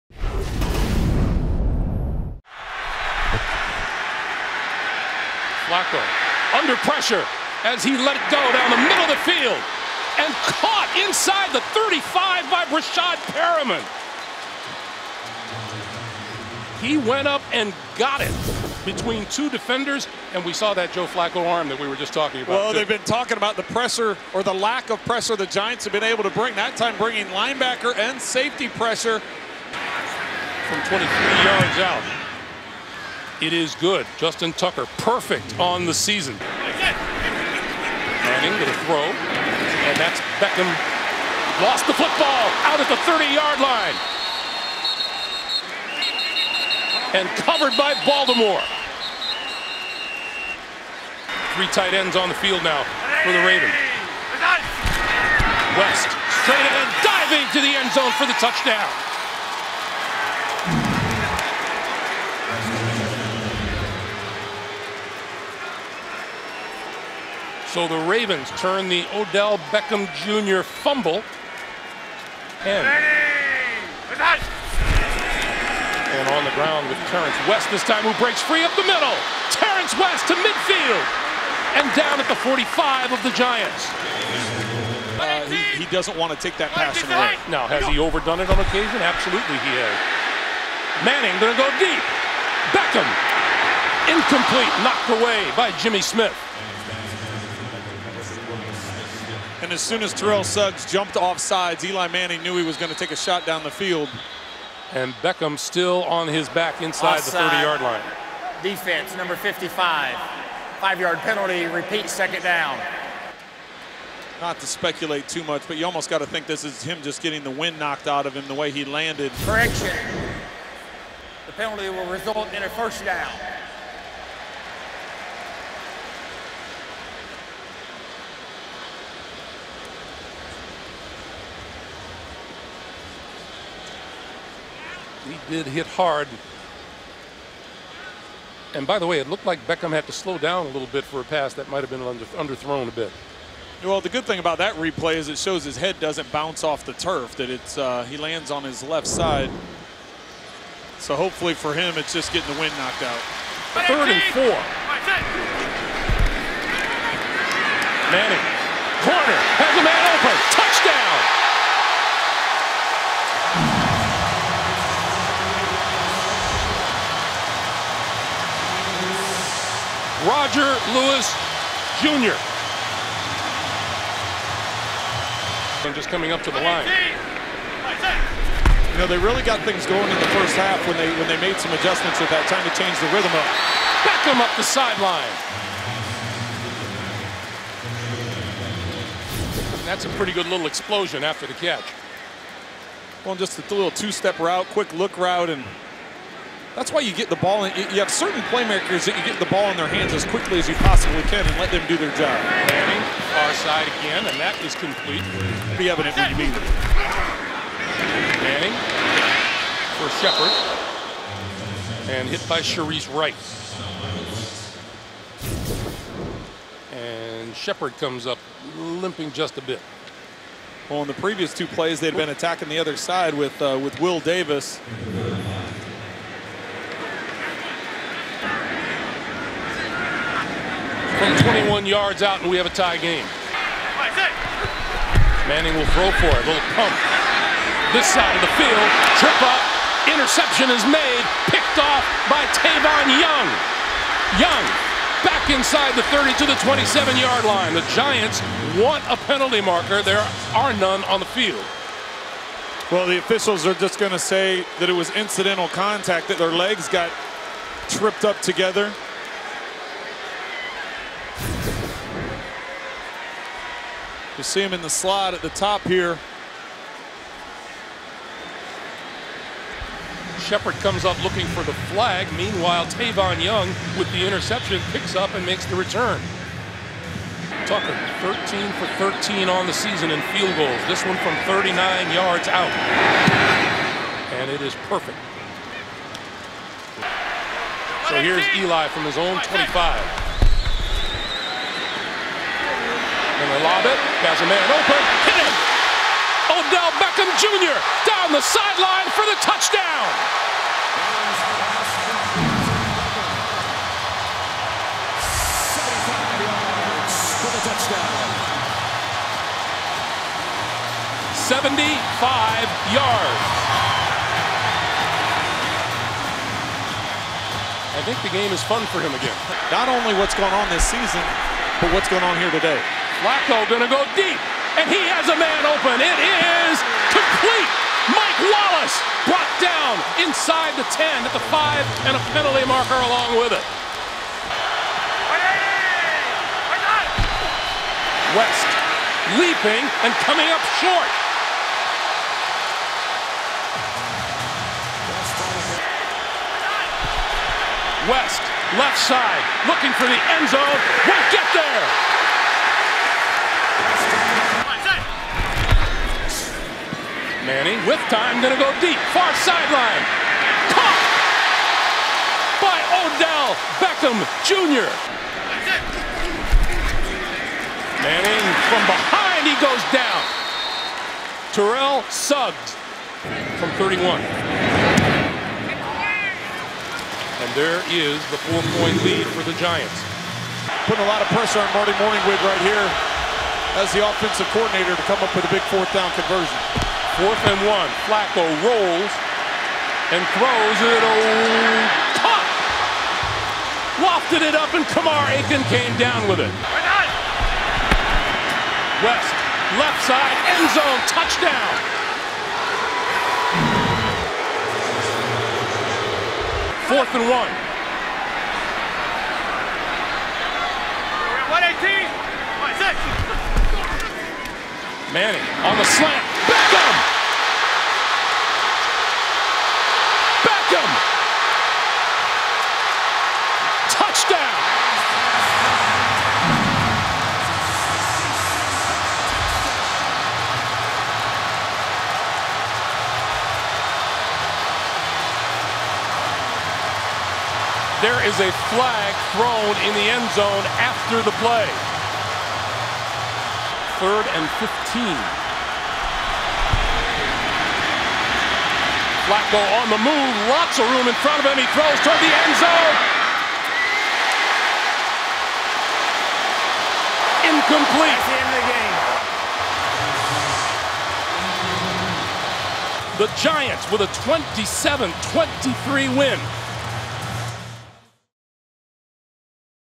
Ravens vs. Giants | NFL Week 6 Game Highlights
A banged-up Odell Beckham Jr. turned a short 4th & 1 pass into a winning 66-yard catch and run with 1:24 to play Sunday as the New York Giants snapped a three-game losing streak with a stunning 27-23 victory over the hurting Baltimore Ravens.